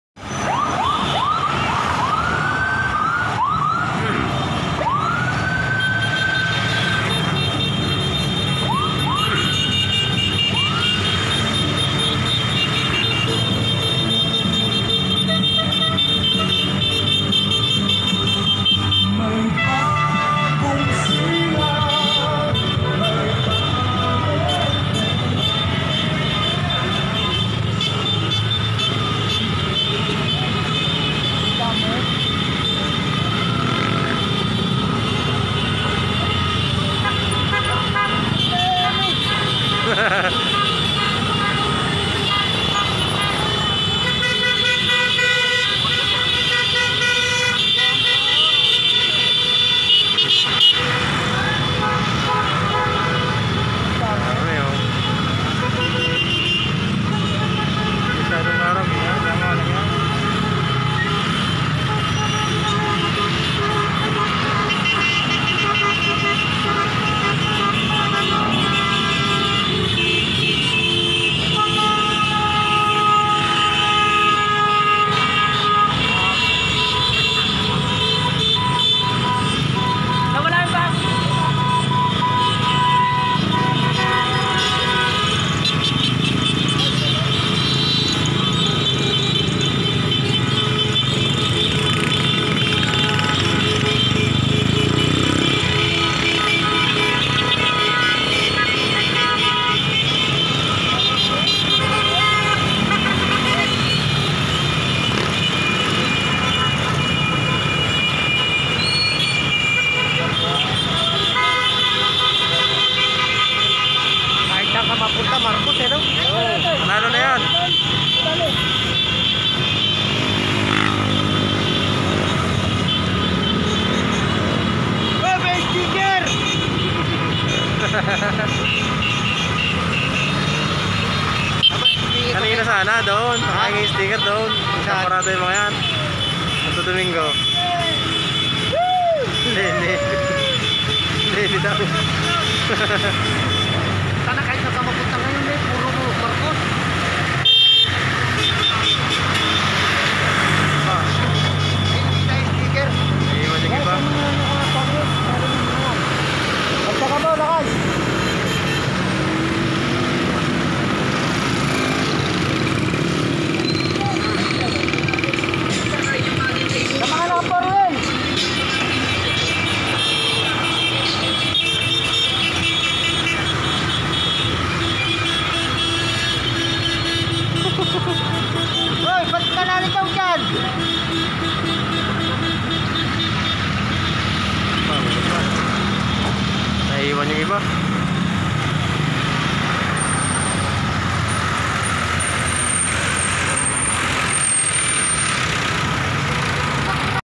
Solid BBM for fresident.sta rosa parade.
Maiba muna tayo mga ka hunter.sana po ay wag tayung magpapa bayad sa mga naninira.kung Anu Ang nasa puso natin yan ang sundin natin sa tulong at away ng Dios.. BBM for fresident.